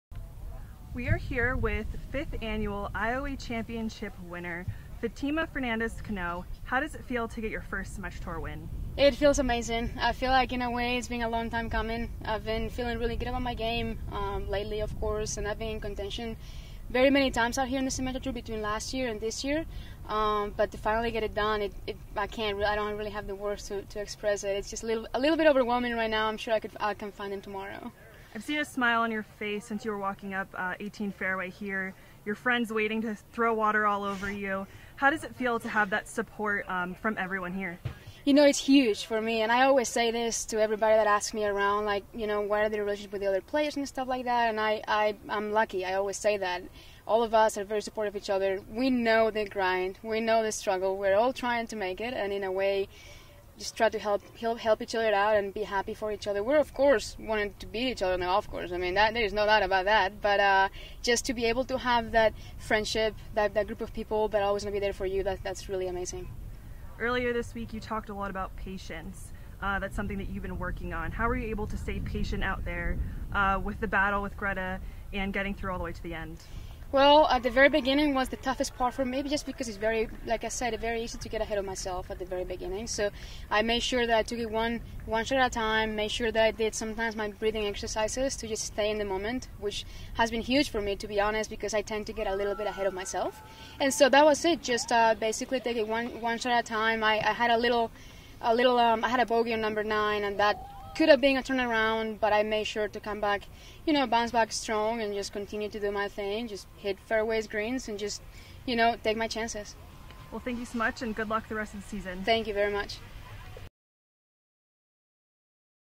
IOA Championship Winner's Interview - Fatima Fernandez Cano
Reaction from Troy University alumna Fatima Fernandez Cano after winning the fifth annual IOA Championship presented by Morongo Casino Resort & Spa hosted by Morongo Golf Club at Tukwet Canyon